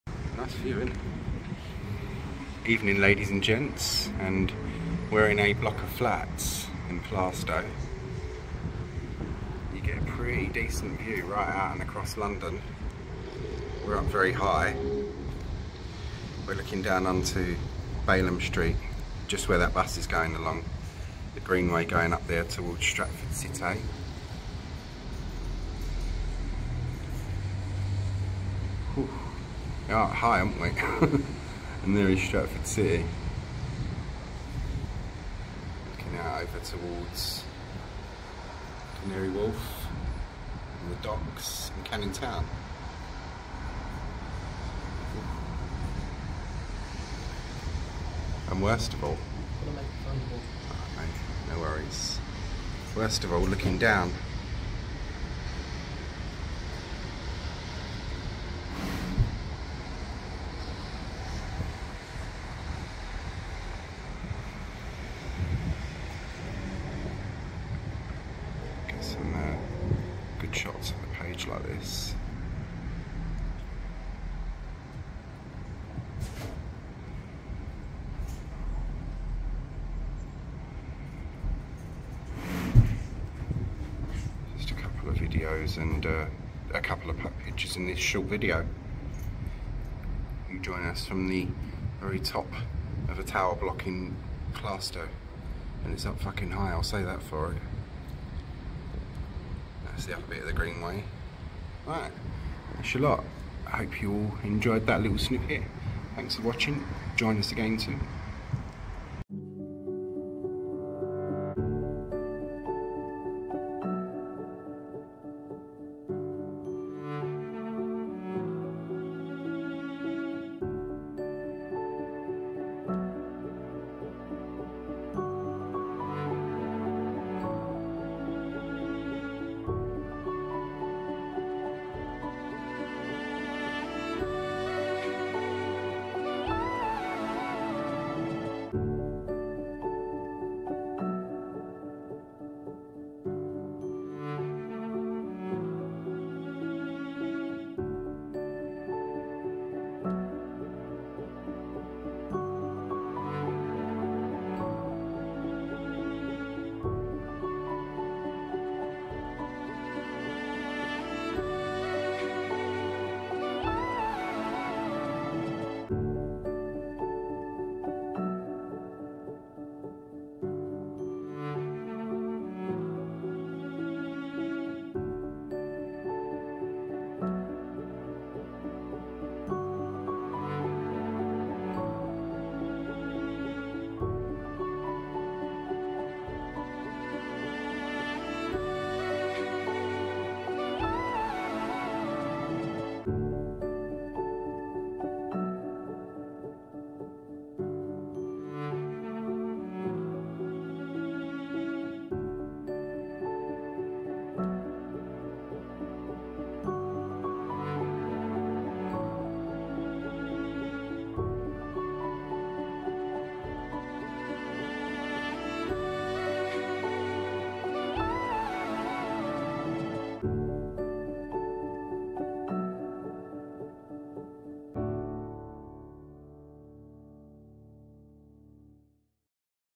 Bird's-eye view from a Plaistow Tower Block. See description ⬇️
Aerial views = from a Tower Block (Bemersyde Point) in Plaistow London E13. Filmed & photographed, on this drizzly evening = Saturday 22nd July 2023.

Ps. We gained this view, from a widow on the top floor, in one of the service areas 🔑🗝😉👌